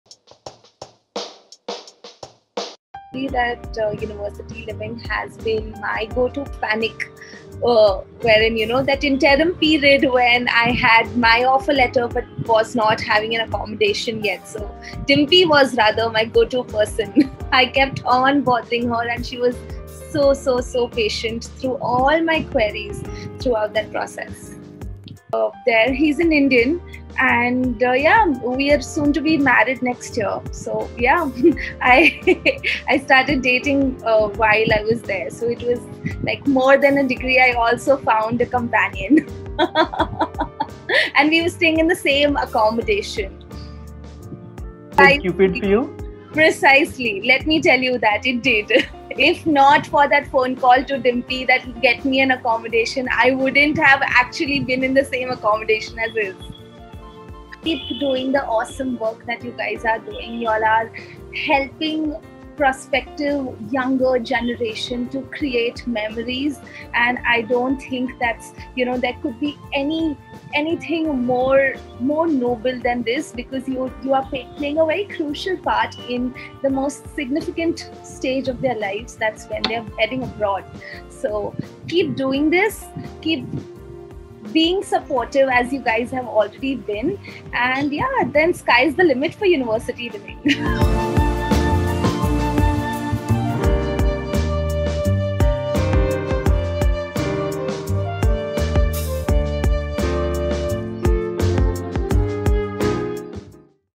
Student Review
Enhancing the overseas education journey, one student at a time! It's so heartwarming to know that our hard work leads to something so beautiful. Hear what Dhwani has to say about us!